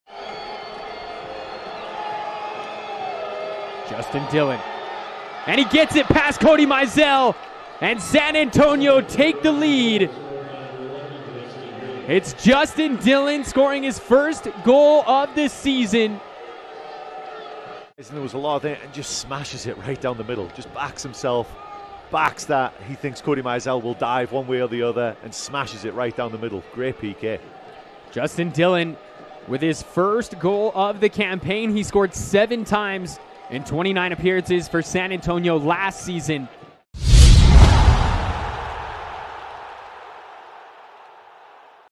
Justin Dhillon with a Penalty Goal vs. New Mexico United
Justin Dhillon (San Antonio FC) with a Penalty Goal vs. New Mexico United, 04/23/2022